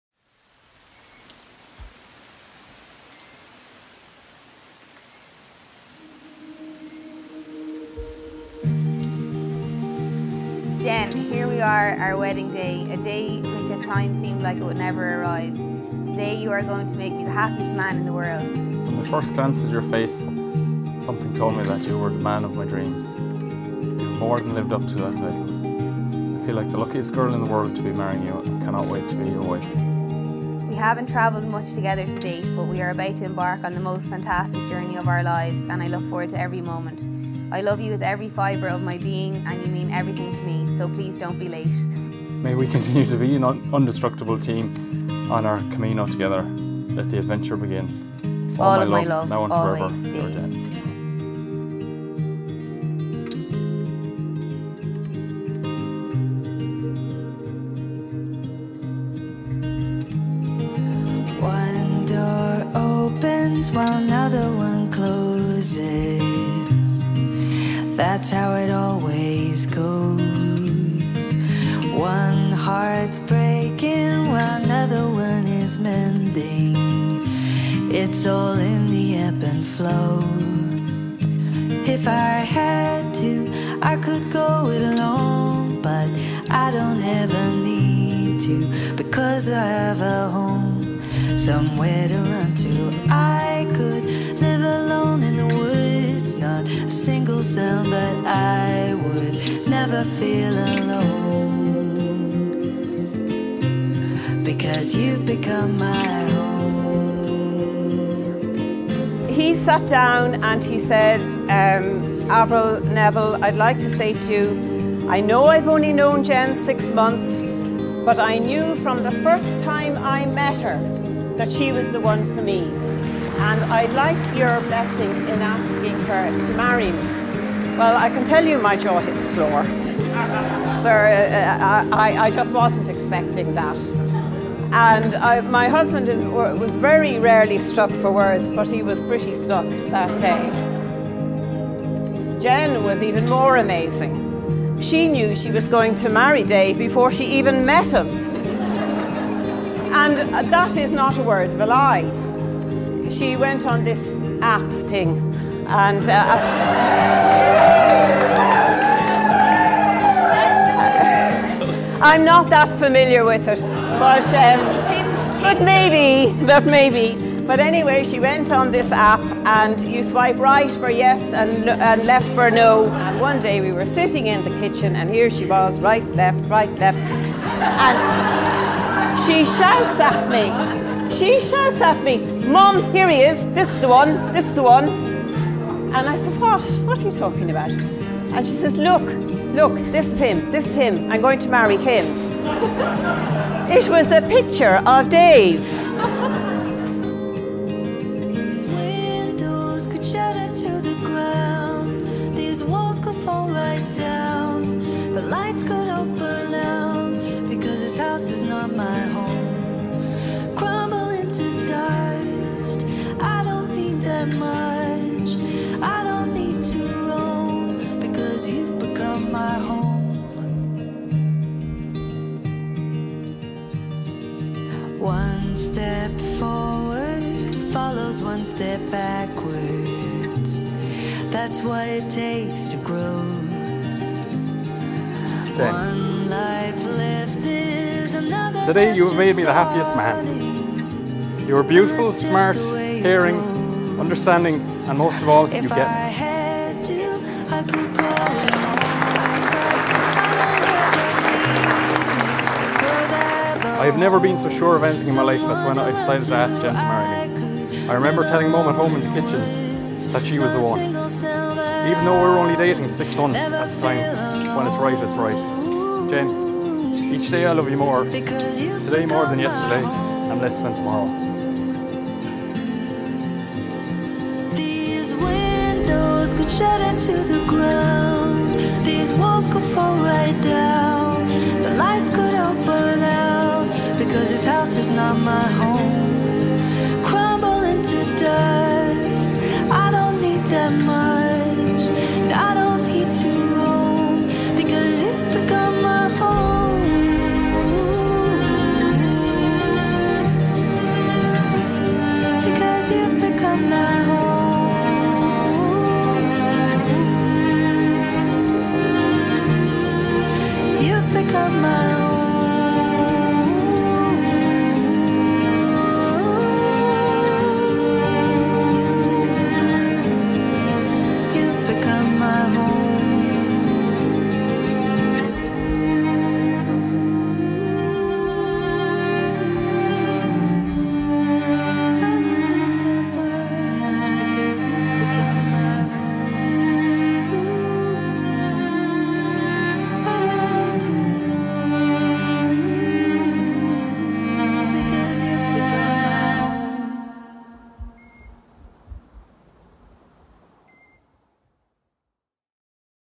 The Green Barn - Jennifer and David Highlights Film
A highlight from Jennifer and David's Big day at the beautiful and quirky venue The Green Barn.